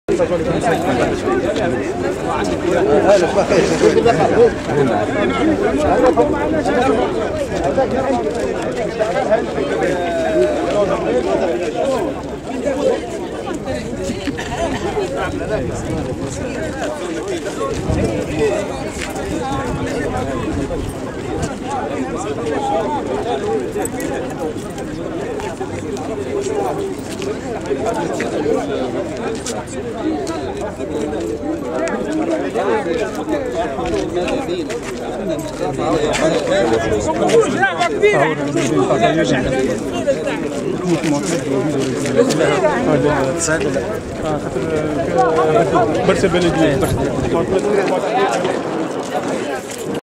زيارة ميدانية لقنال سيدي عبد السلام و ميناء الصيد البحري بصيادة
الجمعة 2017/08/18
بحضور :
والي المنستير
الرئيس المدير العام للديوان الوطني للتطهير
المدير الجهوي للتجهيز
المدير الجهوي للفلاحة و الموارد المائية والصيد البحري
وكالة حماية و تهيئة الشريط الساحلي
نواب عن الجهة